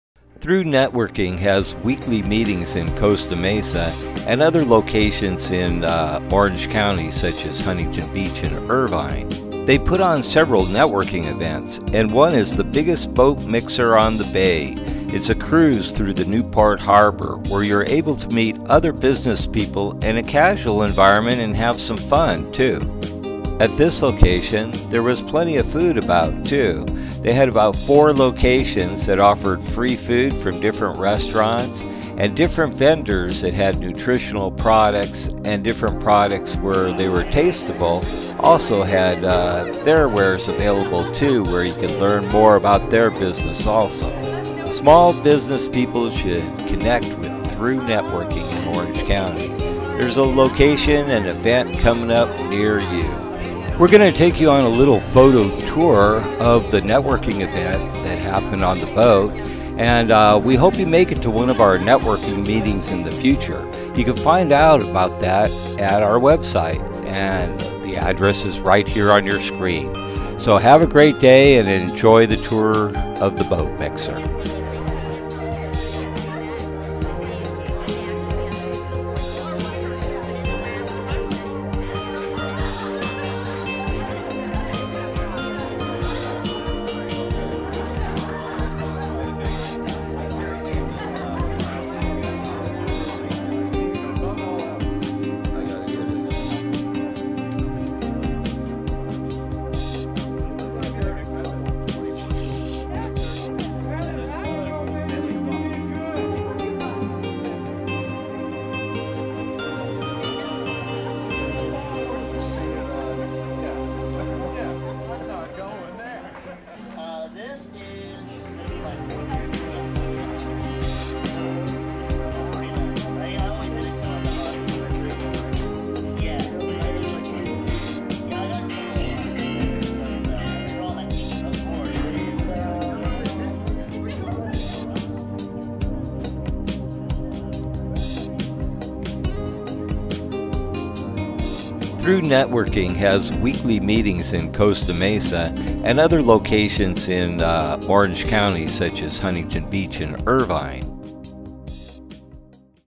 Tru Networking Biggest Boat Mixer on the bay
Tru Networking for it's 4th year is doing the biggest networking event on the bay. We have rented the biggest boat in Newport Beach called the Eternity. With 4 levels.. Networking meeting happen weekly in Orange Count. City's that we have events currently are Costa Mesa, Irvine and Huntington Beach. What makes our group so great is the net workers that help and participate in the events and mixers.